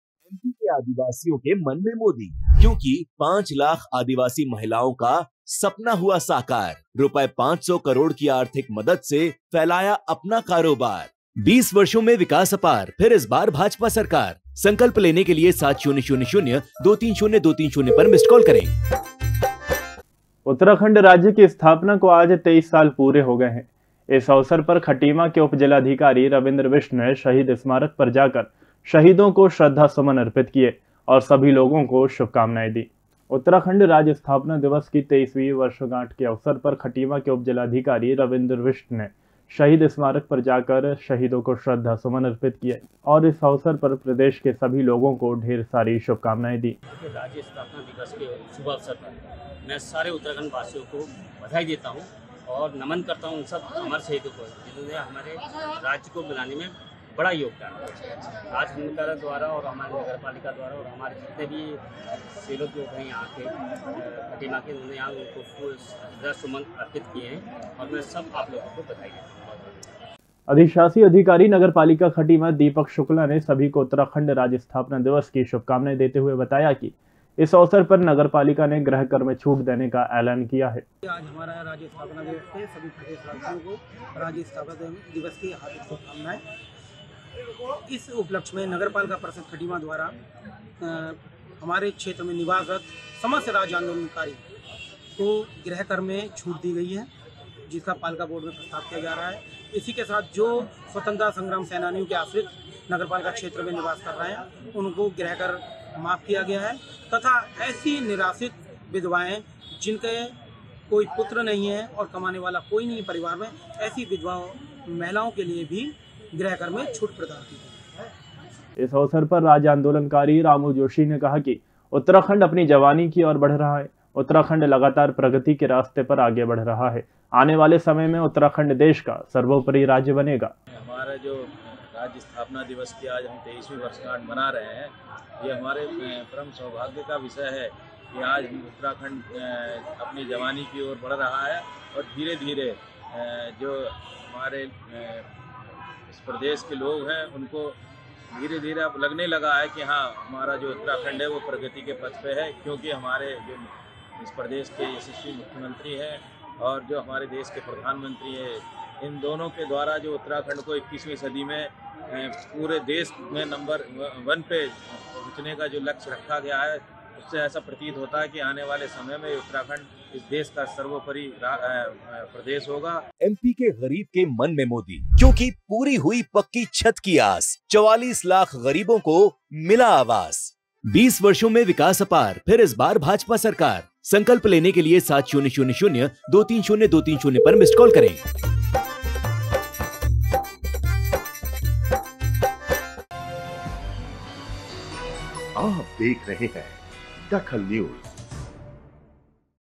DAKHAL NEWS उत्तराखंड राज्य की स्थापना को आज 23 साल पूरे
उत्तराखंड राज्य की स्थापना को आज 23 साल पूरे
रविंद्र बिष्ट ने शहीदों को श्रद्धा सुमन अर्पित किये